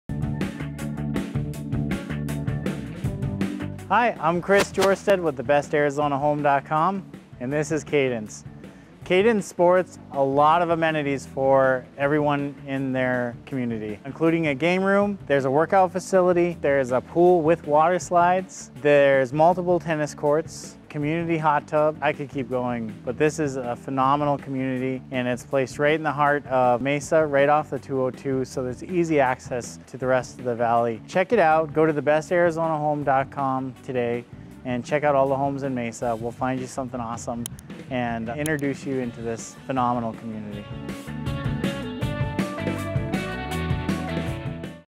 Cadence at Gateway | AZ 2022
Cadence at Gateway is a one-of-a-kind neighborhood. It’s filled with amenities including a full fitness center, resort-style pools, a community center, an indoor game space, soccer, volleyball, bocce, and basketball courts, sport fields, shaded play areas, ramadas, a dog park, and a café-style hangout. Cadence is one of the best Mesa, Arizona has to offer, with a new Silver Valley elementary school within the city, a brand-new high school five minutes north, numerous award-winning builders, and over 50 floor plans to choose from. Cadence at Gateway primarily consists of midsize homes that are reasonably priced. This is a long-established neighborhood that continues to pique the interest of buyers in the Mesa area.

Check us out on F A C E B O O K